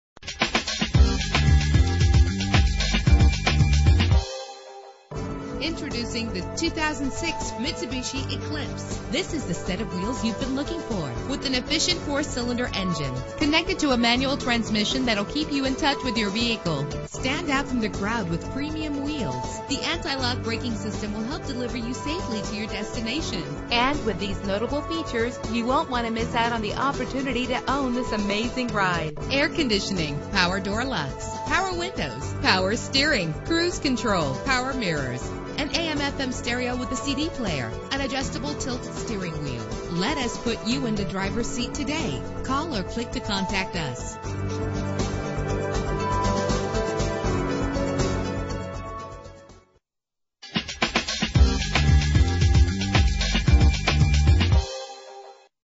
2006 Mitsubishi Eclipse - Nashua NH
Year: 2006
Make: Mitsubishi
Model: Eclipse
Trim: GS
Engine: 2.4 liter inline 4 cylinder 16 valve
Transmission: 5-Speed Manual
Color: Orange
Mileage: 102749
Address:
3 Marmon Drive